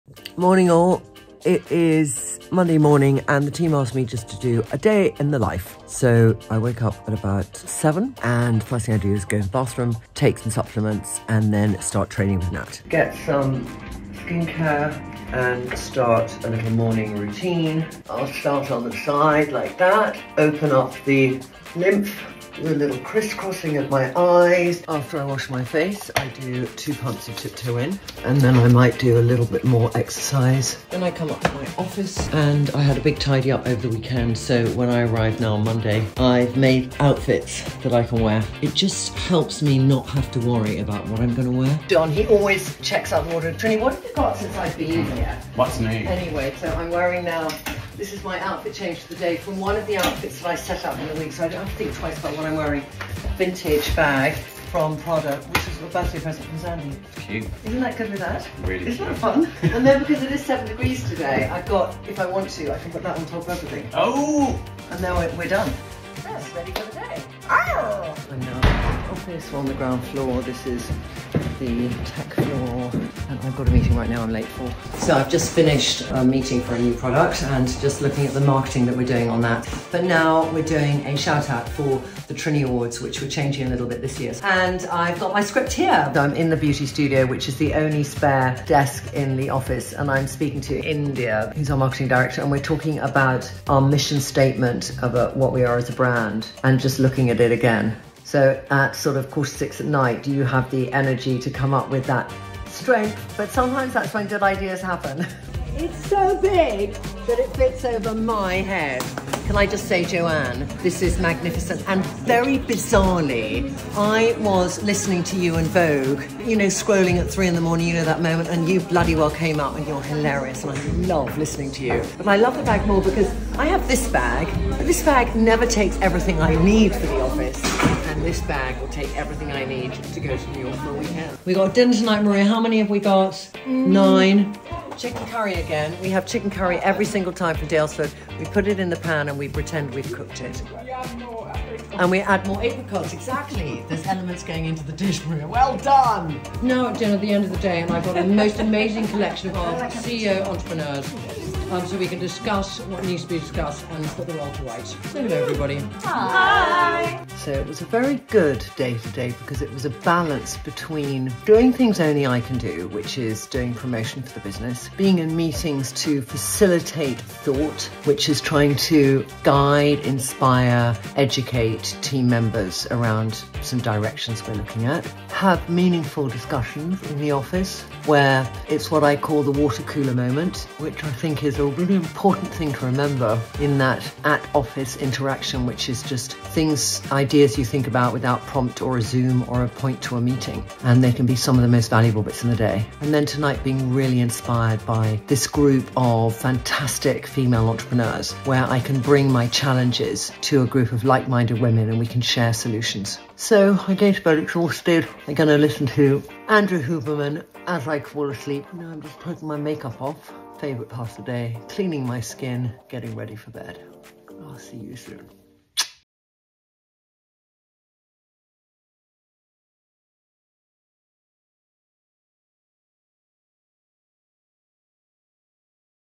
Day In The Life Of Trinny Woodall
One of the questions Trinny is asked the most is 'how do you manage to fit everything in?', so today she’s taking us along for a jam-packed day in her life. From invigorating at-home workouts to meaningful meetings with the team at HQ and inspirational dinners with female entrepreneurs, there are (just) enough hours in the day to find the balance between work and play. And then it’s time to rest, recharge, and get ready to do it all again tomorrow…

Makeup and skincare all Trinny London.